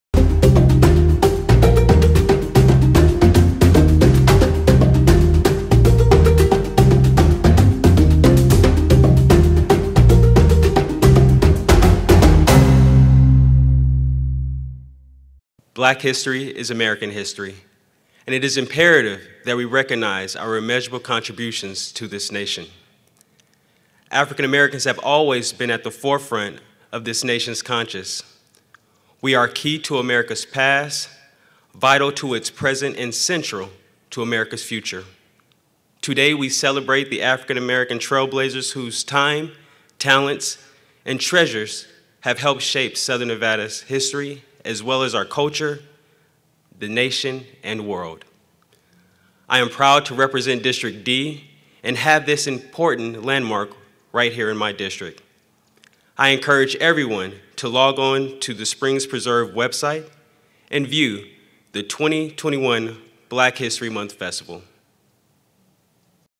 A word from Commissioner William McCurdy II
A word from Clark County Commissioner William McCurdy II representing District D

Explore more videos and content from the Springs Preserve's 2021 virtual Black History Month Celebration